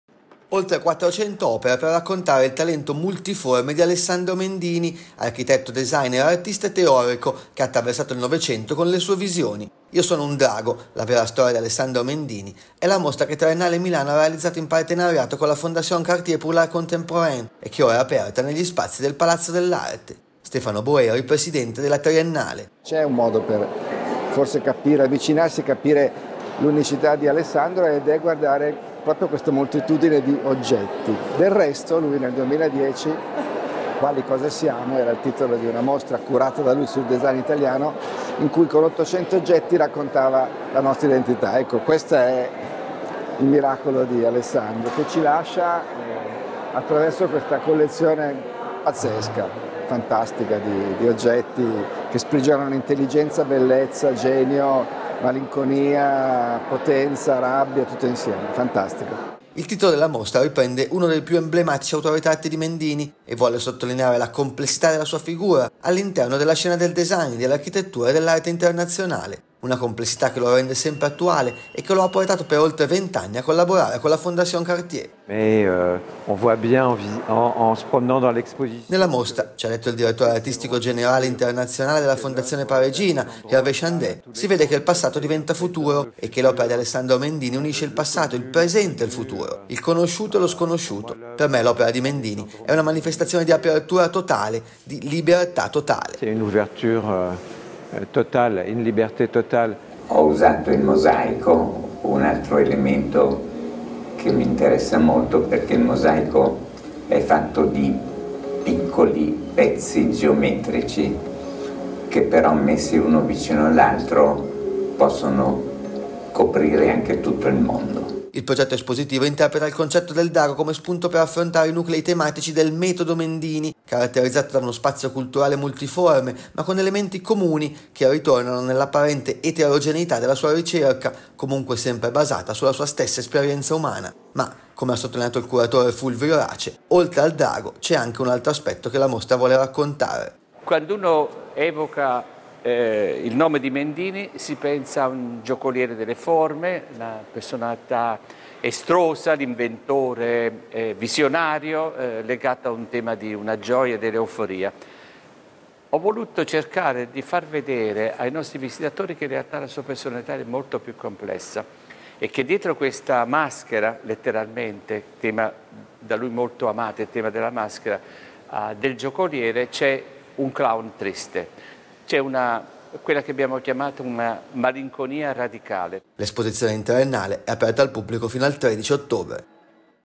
Tutti i volti di Alessandro Mendini, Triennale celebra un "drago"
A Milano la mostra realizzata con la Fondation Cartier